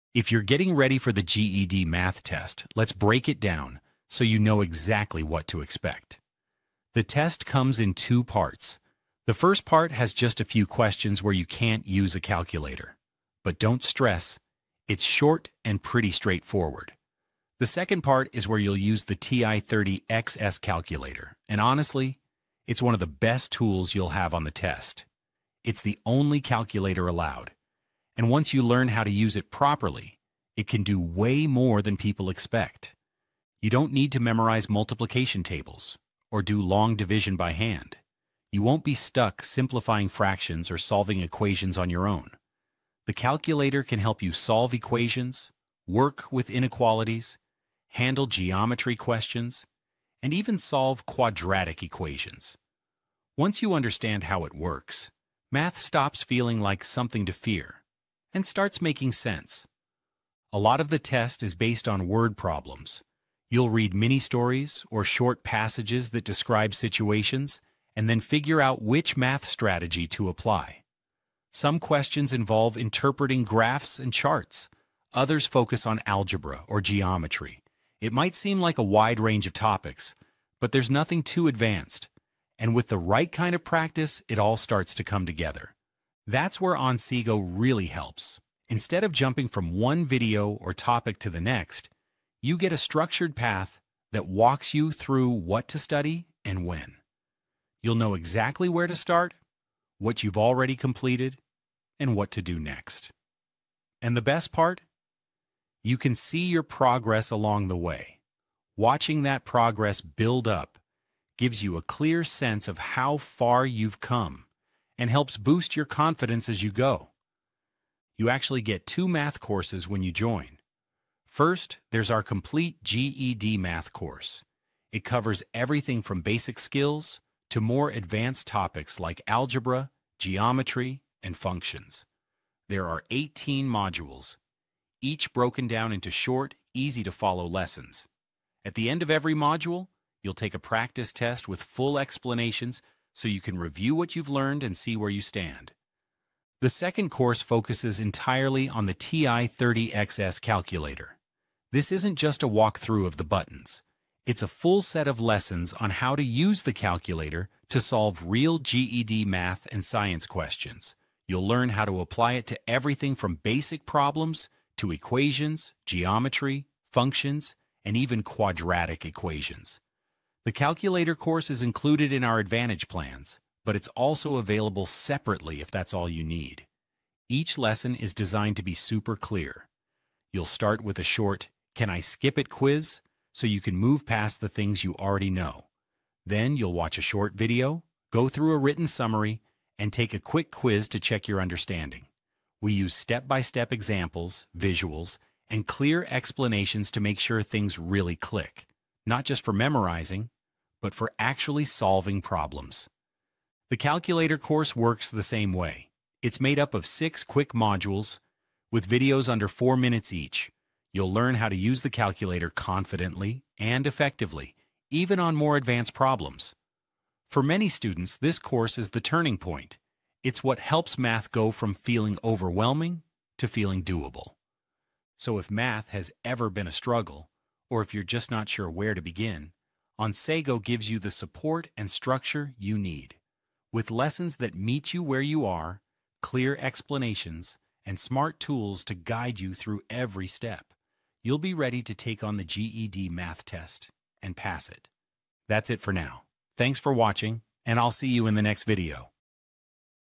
Studying GED Math with Onsego
If you’re studying GED Math and are not sure where to start, this video is for you.
We explain what’s on the GED Math test, which topics are most important, and how you can build the skills you need—one step at a time.